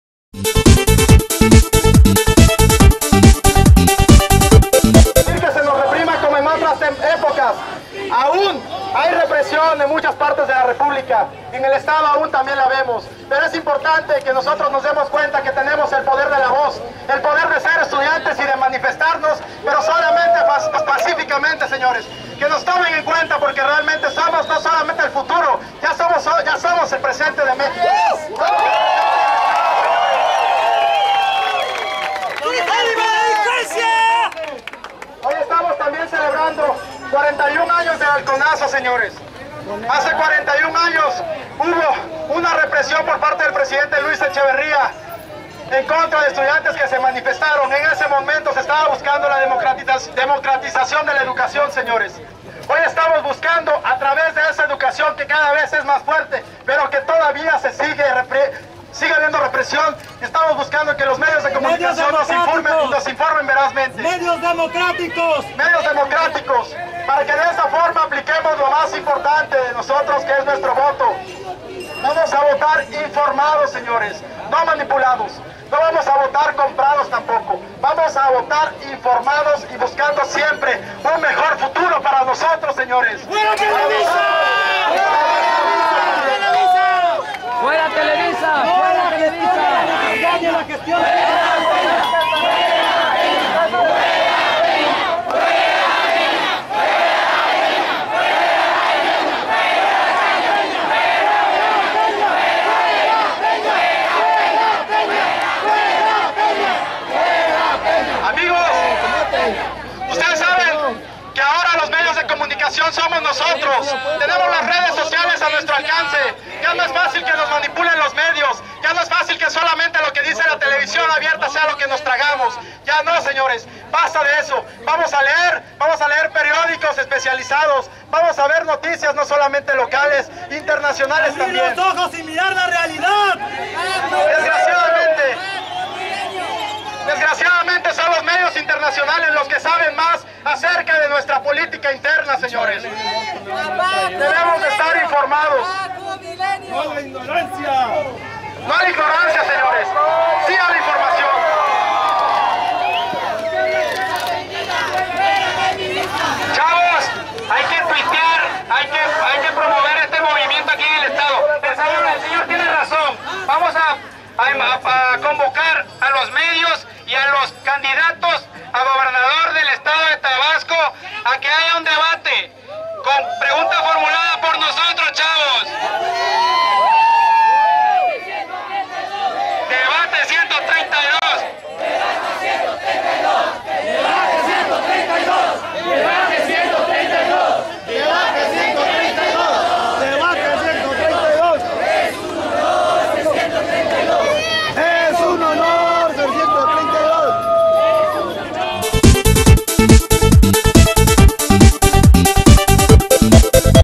Llegada a Plaza de Armas y palabras de Amado Cabrera desde el templete yosoy132.mpg
Llegada a Plaza de Armas de la marcha por la libertad de expresión del movimiento estudiantil Yosoy132 en Villahermosa Tabasco el de 10 junio de 2012 con la participación de las principales universidades privadas y la del estado. En dirección a la Plaza de Armas donde se concentraron para organizar los camités de cada Universidad. Discurso de Amado Cabrera.